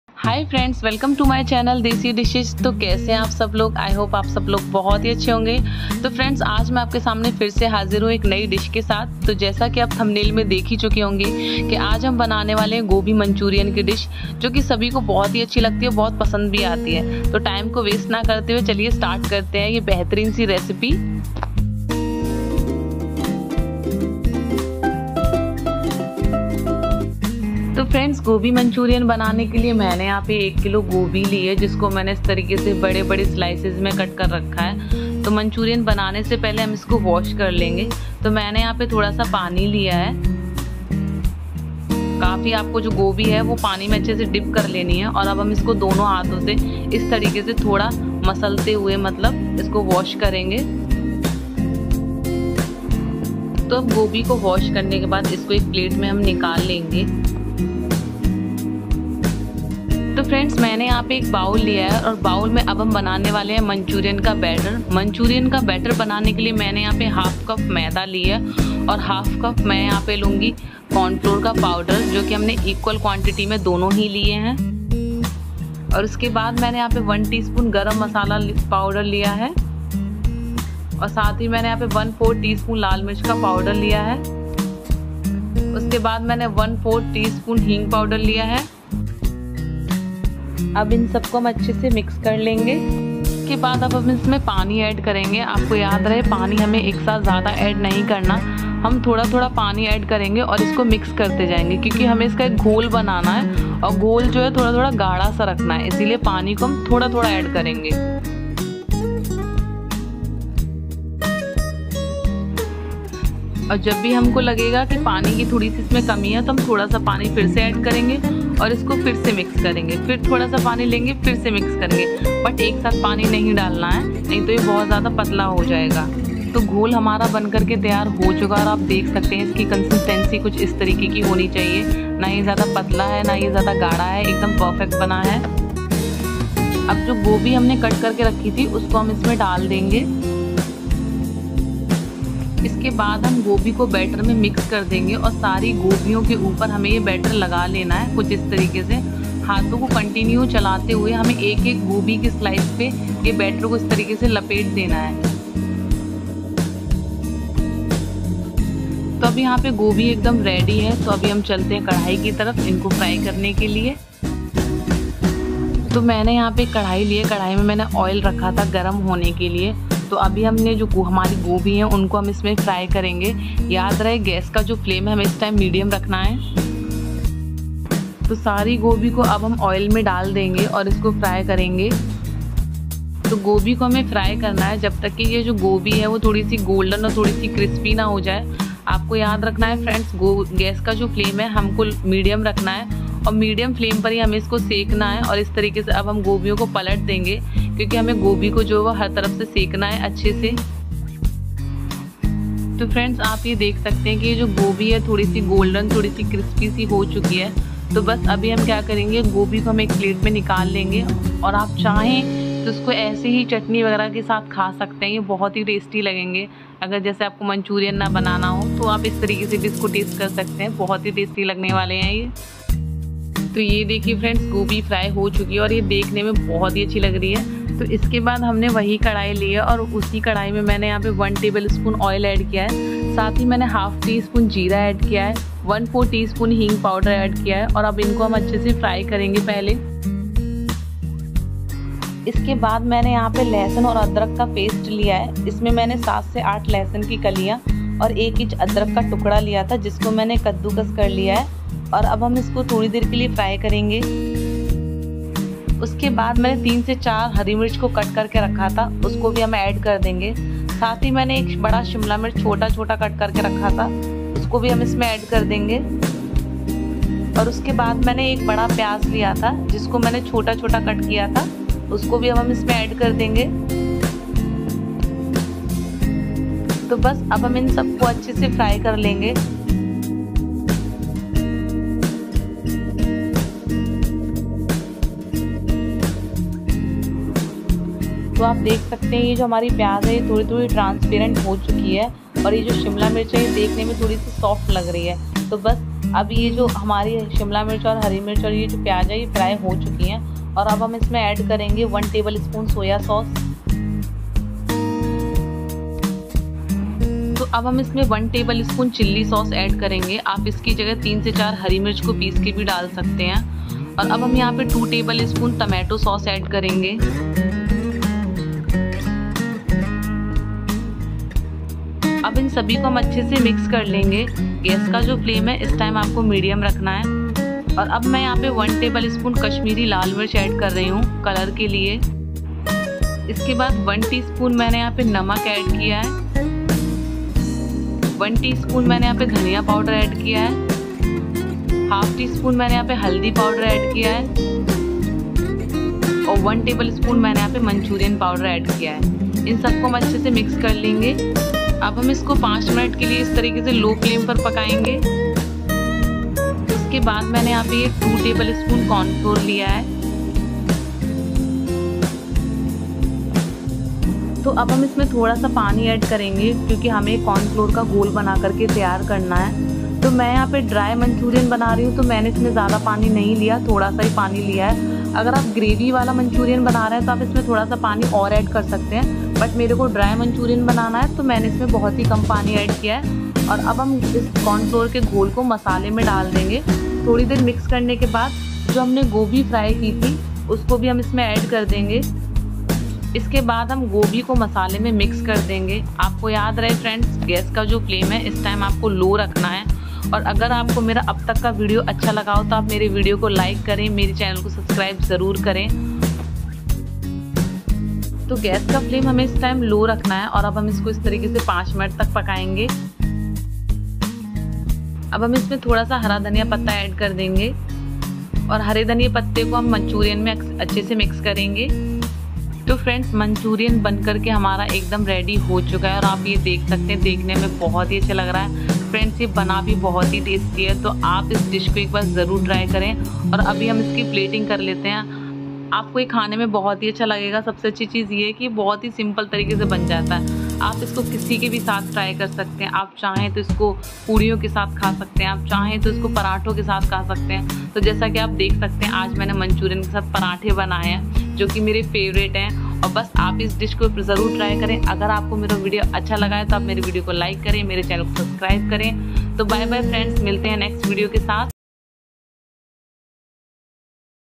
Gobi Manchurian Recipe|Crispy Manchurian Recipe| Gobi Manchurian Dry
For better
1/2 cup maida
1/2 cup corn flour
1 tsp garam Masala
1/2 tsp Lal mirch
1/2 tsp hing powder

For manchurian
1 kg cauliflower
1 tbsp oil
1/2 tsp jeera
1/3 tsp hing
1tbsp soya sauce
1tbsp chilli sauce
2 tbsp  tomato sauce
1tbsp Kashmiri Lal mirch
1 tsp namak
1 tsp coriander powder
1/2 tsp terminuc powder
1tbsp manchurian powder